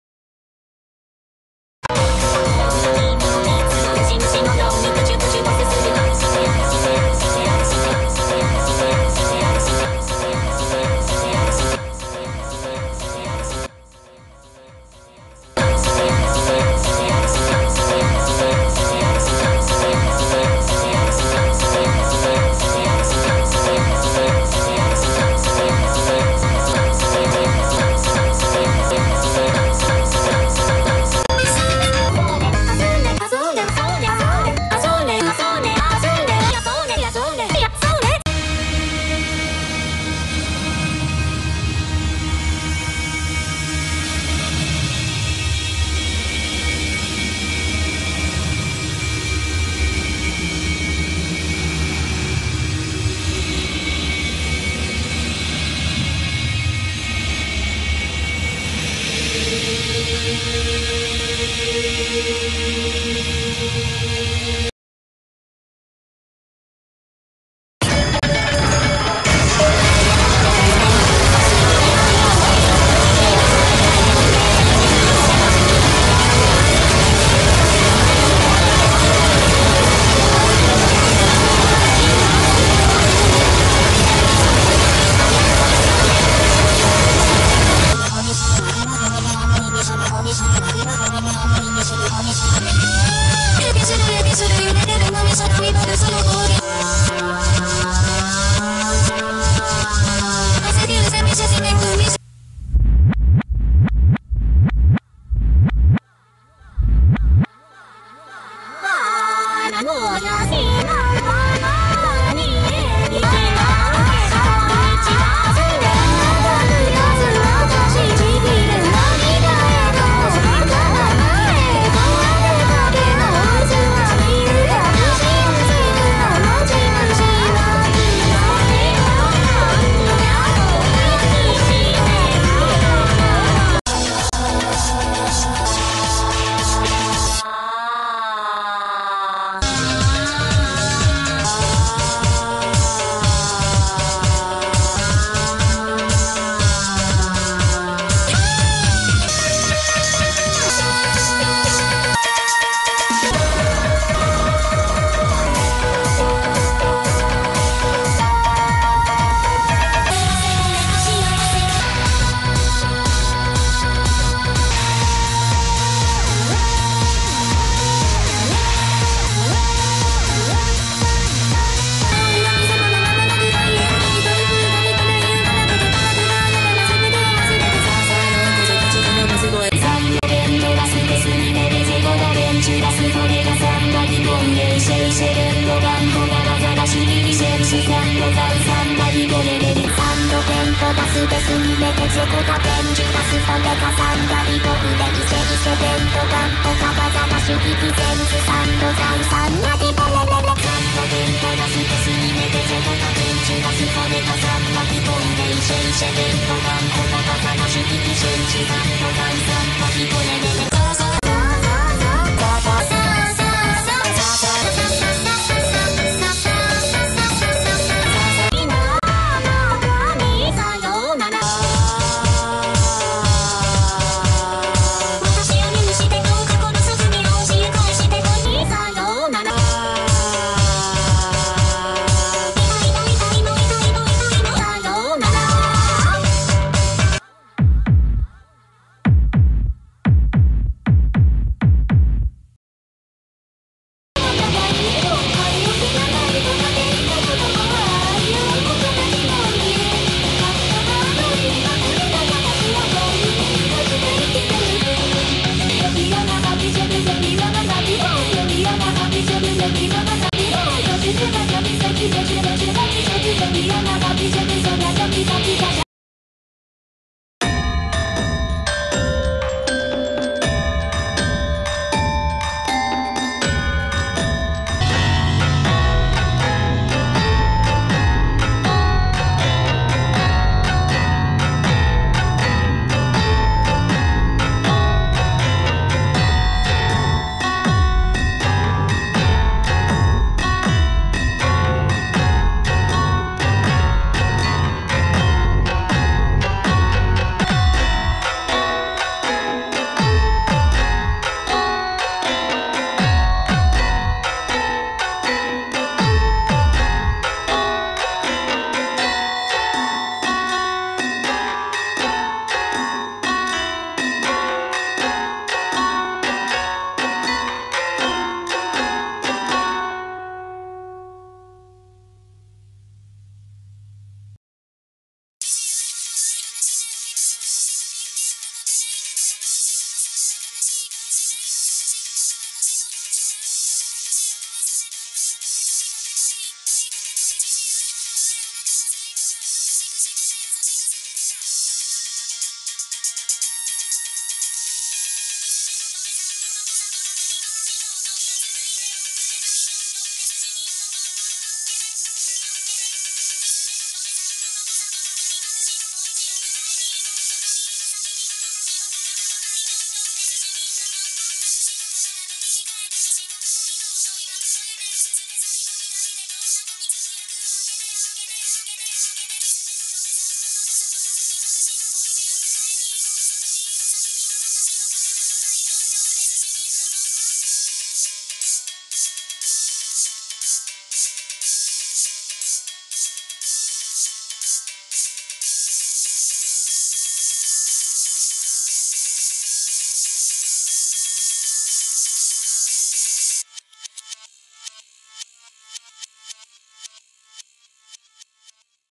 a collection of random masa related things ive made
most of these are really old now. ahh memories

the asore compilation, screaming compilation, saa compilation, and sisters story edit are the oldest things in this video, I believe